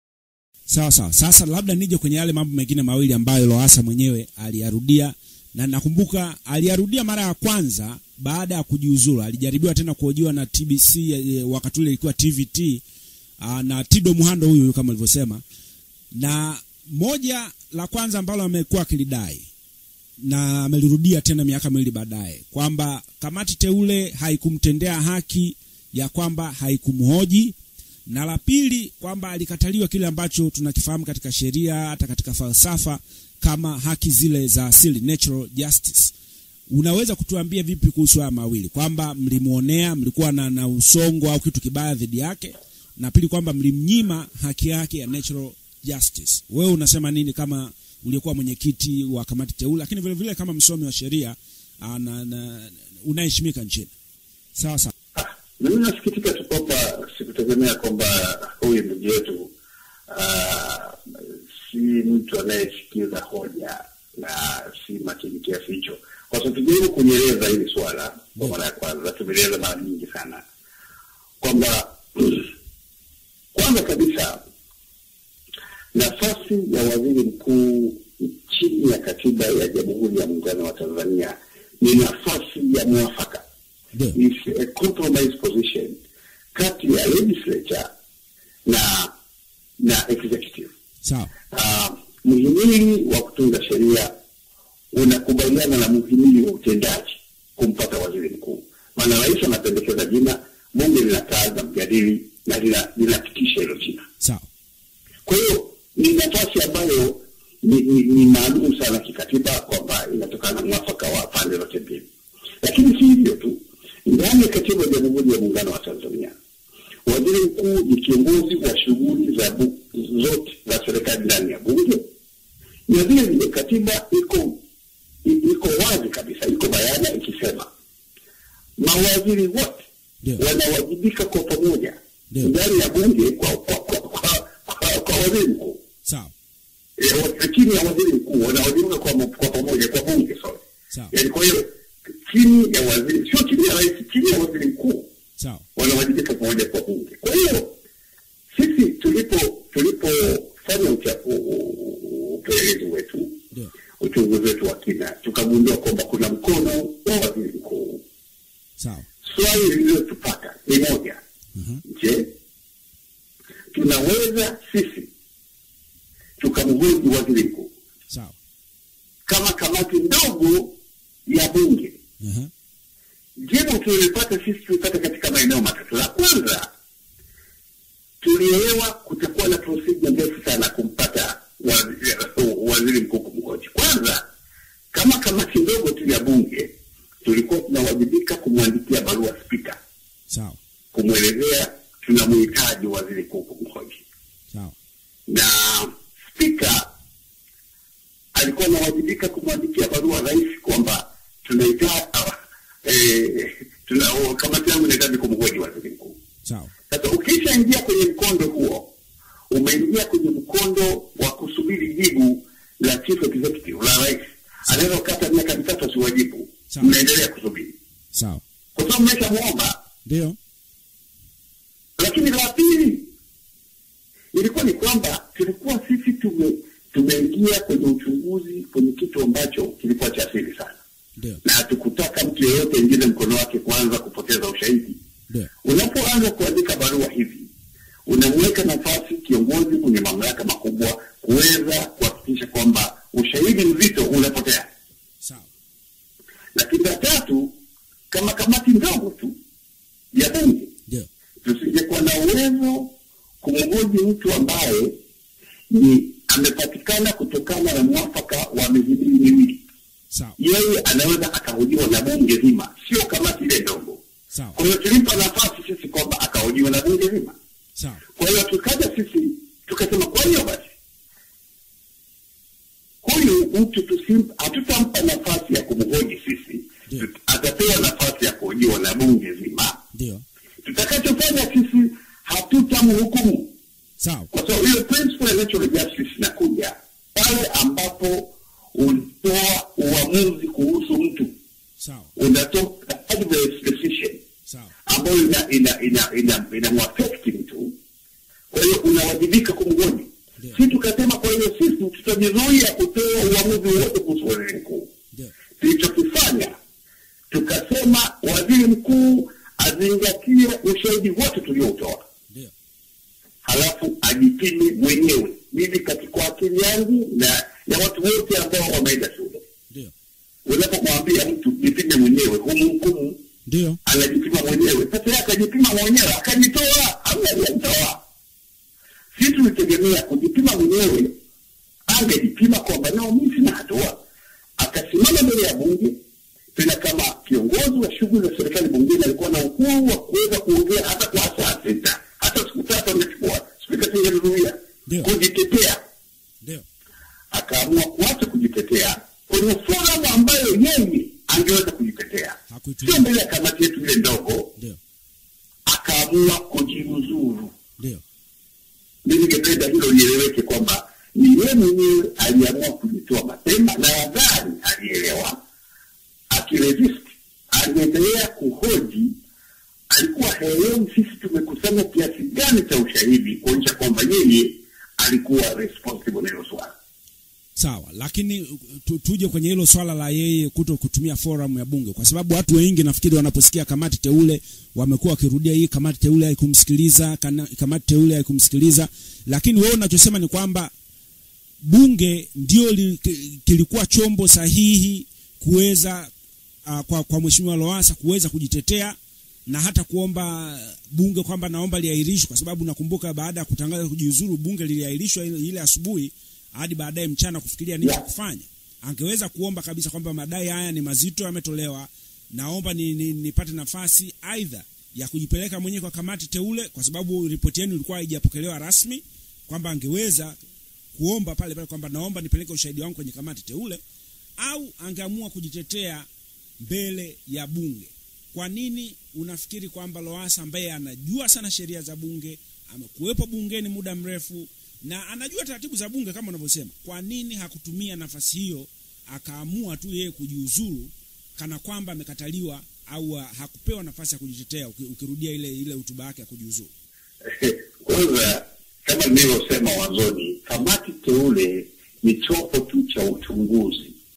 MAJIBU YA DR. MWAKYEMBE-2
Hii ni sehemu ya pioli ya majibu ya Dr. Mwakyembe juu ya hoja zilizotolewa na Mhe. Edward Lowassa, Waziri Mkuu wa zamani.